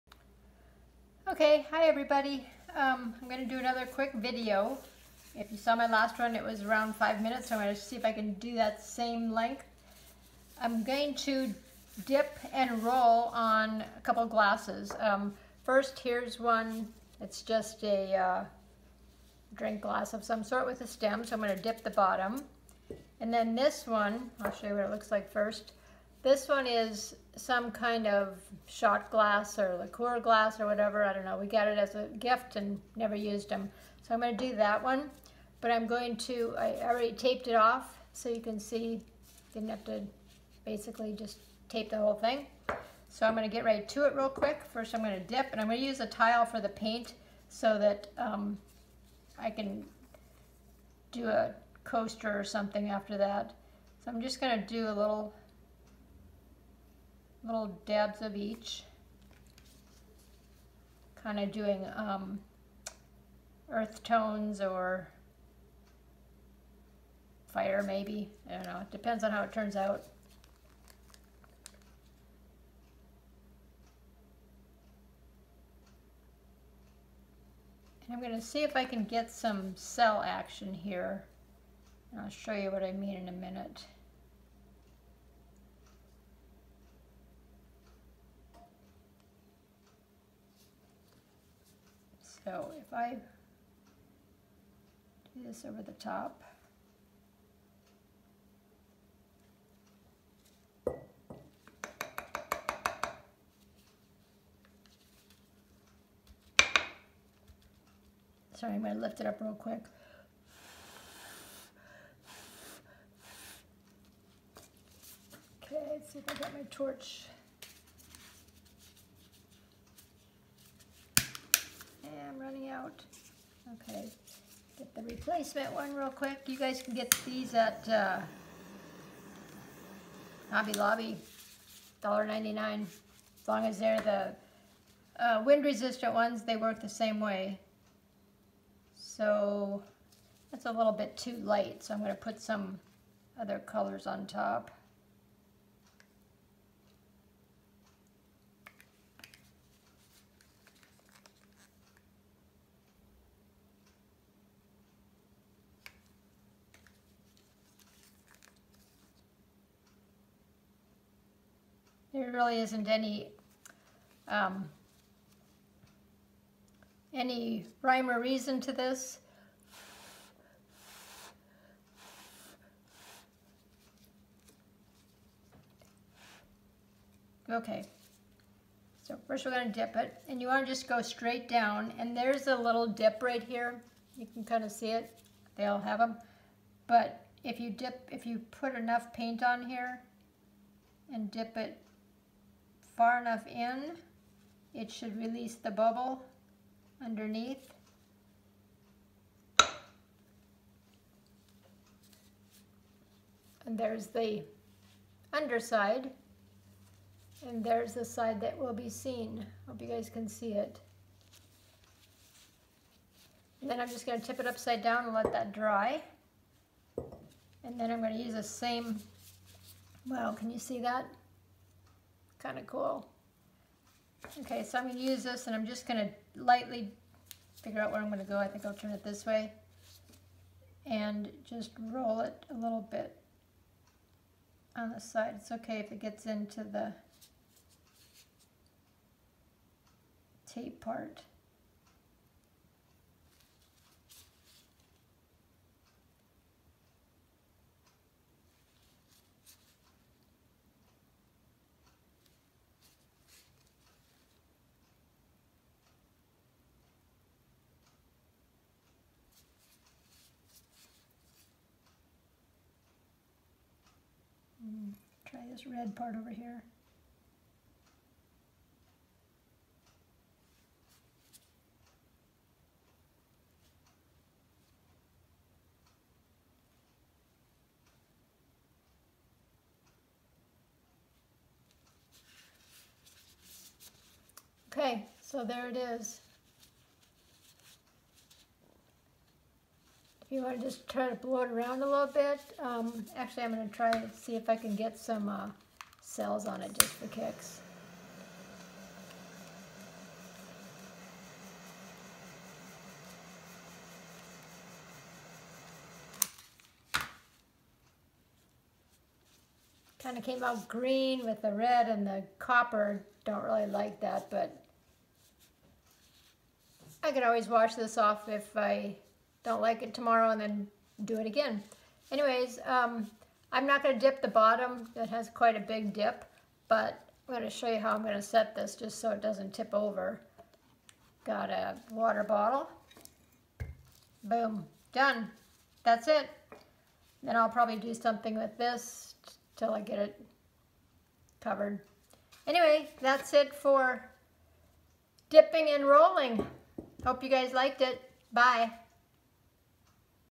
Acrylic Pouring - Simple Dip Technique on Wine Glasses
Inspired by Trees Marie who does amazing pouring on glass.

You can customize the underside of your stemmed glasses with this simple dip technique or do a dip & roll on the outside. The dip & roll works best on straight glasses like the shot glass in the second half.

I ended up washing off the shot glass though and poured different colors on that one. The new purple, white and black color combo is much nicer than the earth tones for this type of glass. Video is